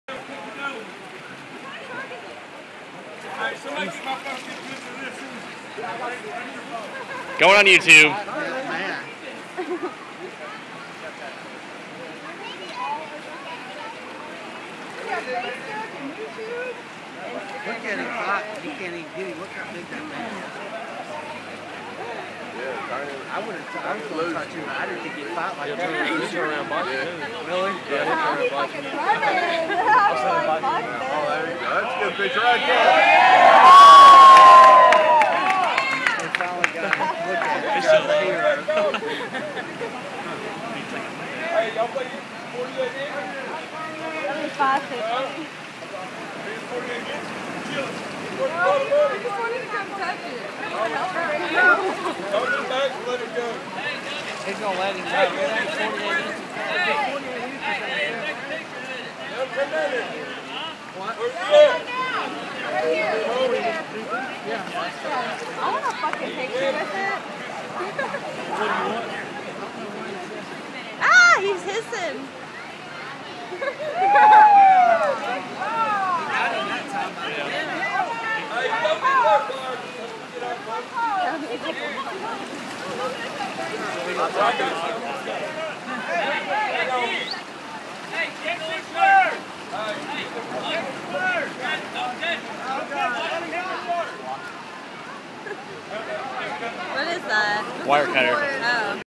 Shark attack!
A caught shark in PCB. I don't know. It was cool. Brentwood Baptist Church Senior Stay